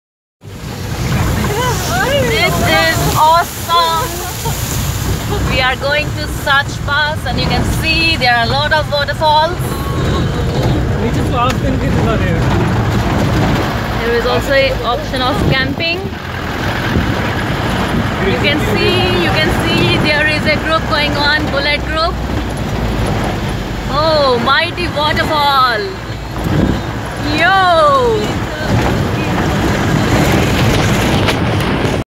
Sach Pass - Bullet Riders Paradise
Sach Pass in District Chamba of Himachal Pradesh... Pass leading to Killar Pangi Valley